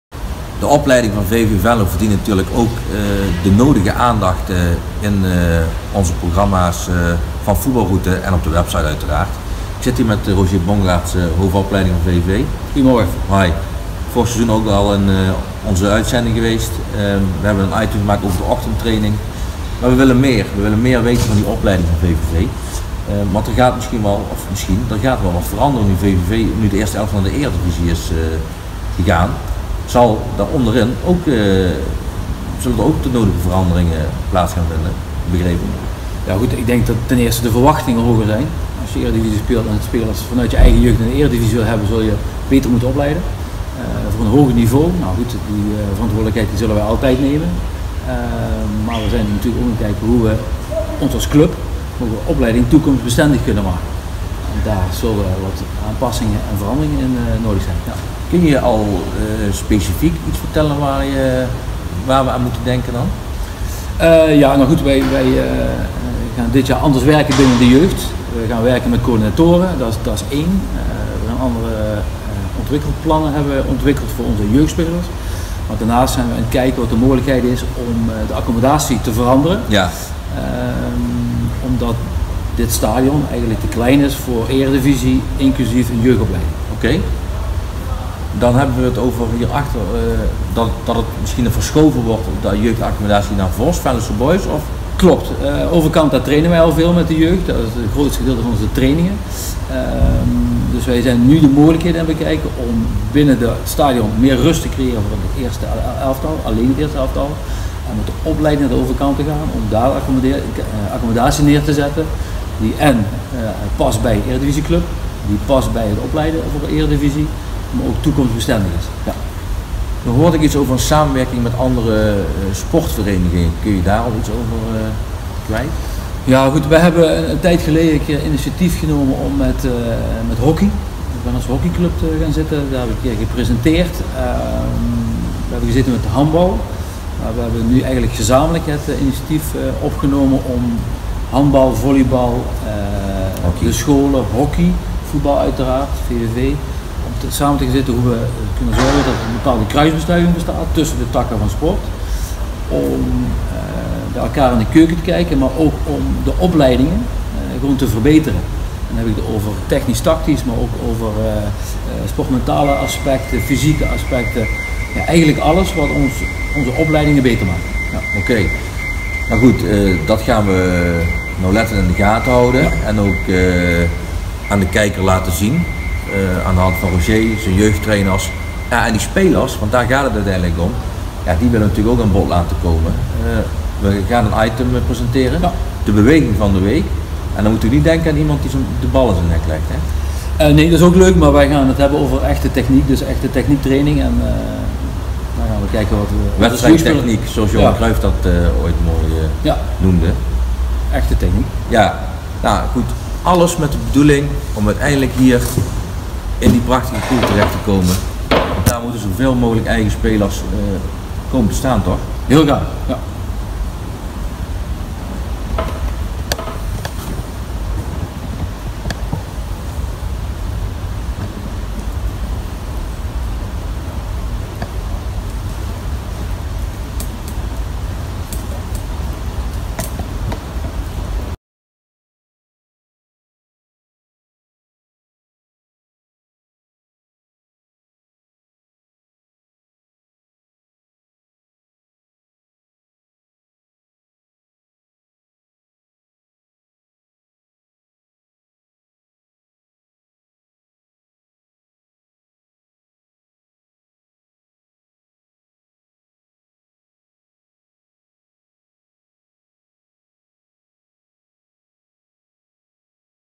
Aanstaande zaterdag in Food Bal Route vanuit Leonardo mode voor mannen
Regisseur: Jules Hendriks
Presentator: Qasim Hakim
Vaste tafelgast: Hay Janssen (raadslid PvdA)
Special tafelgasten: Roger Bongaerts (hoofd opleidingen VVV) en Dominik Vergoossen (hoofdtrainer VVV onder 19 en van eersteklasser Wilhelmina’08).

De kunstgrasdiscussie heeft wat stof, maar vooral korrels doen opwaaien de laatste dagen. Zeker na de wedstrijd VVV-Ajax van afgelopen zondag waarna met name Ajax-coach Marcel Keizer, Klaas-Jan Huntelaar en Volkskrant-journalist Willem Vissers zich zeer negatief uitlieten over de ondergrond van stadion De Koel.
Roger Bongaerts kwam jongstleden dinsdag met een betoog op Facebook waarin hij oproept te stoppen met de smoezencultuur. Het hoofdonderwerp? U ziet ‘m al aankomen. Juist ja: het kunstgras.
Daarnaast praten met we met Bongaerts en Dominik Vergoossen over de jeugdopleiding van VVV, Venlosche Boys (tegenstander van Wilhemina’08 in de eerste klasse), Frankrijk-Nederland en wordt de FBR Toto van de volgende interland en bekerwedstrijden ingevuld.
Regisseur: Jules Hendriks.
Presentator: Qasim Hakim.
Vaste tafelgasten: Joep Bindels en Hay Janssen .